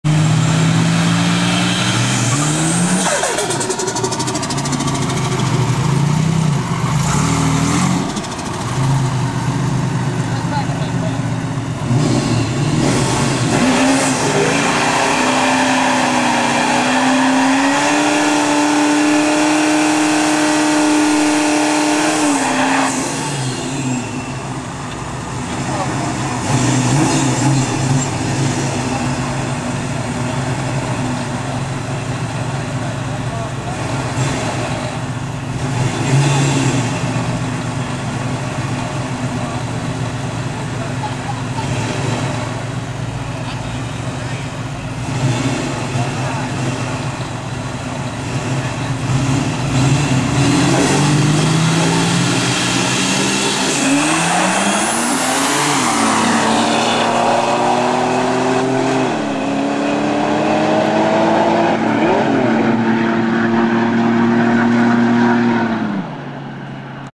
ViciousV6 Vs. S/C Cobra 8-4-07
Out in the woods again! Found a snake that wanted to bite me but instead my tires ran him over!